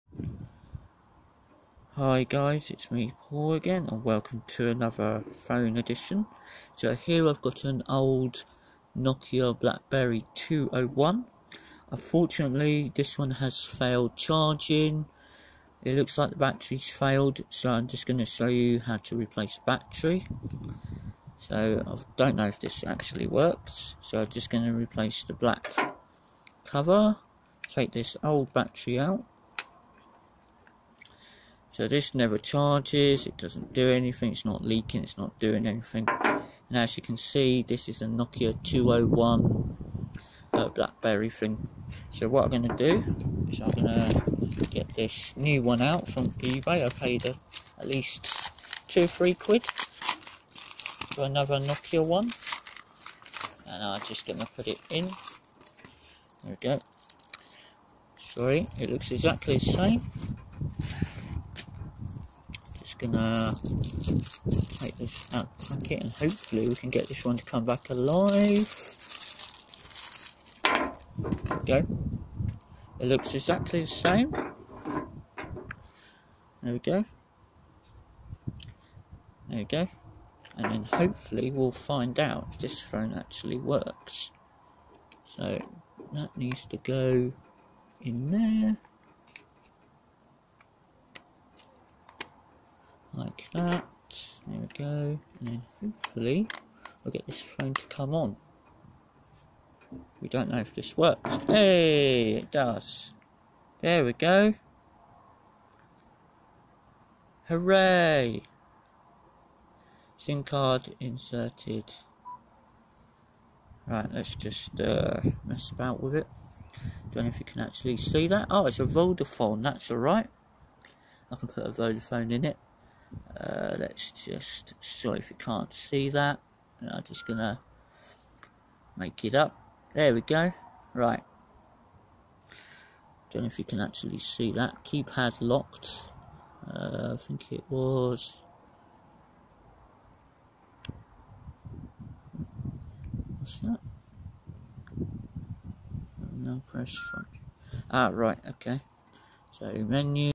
How To Replace Faulty Nokia Asha C201 Mobile Phone Battery 🔋 June 2024
Hi my name is Paul and I have Special Needs Autism and Sensory Needs.

Here is how I replaced a Faulty Nokia Asha C201 Mobile Phone Battery 🔋 and got it working again yay another Phone Saved. Thanks again Paul